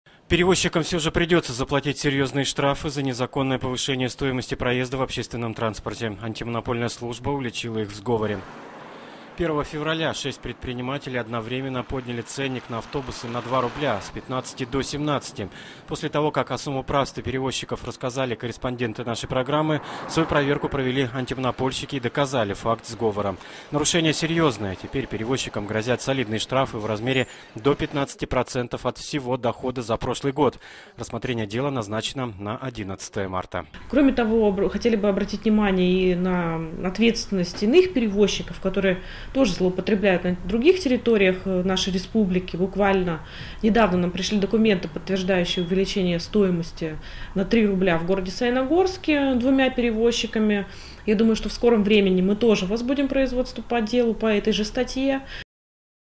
Антимонопольная служба возбудила дело на черногорских пассажироперевозчиков 10.02.2016
Антимонопольная служба возбудила дело на пассажироперевозчиков  из Черногорска